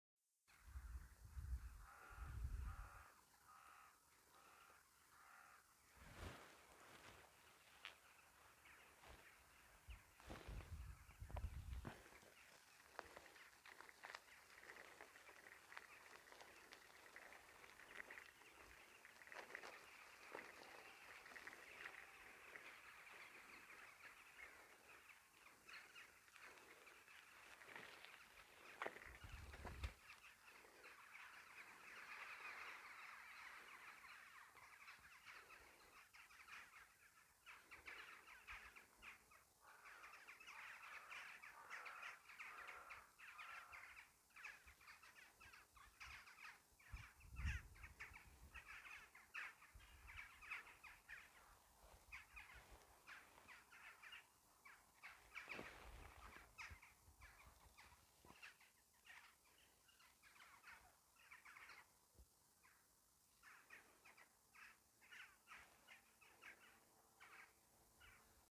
I love crows
While the geese fly over, heading south in the thousands, the crows will stay.
Connecting with "fellow locals" is always a pleasure.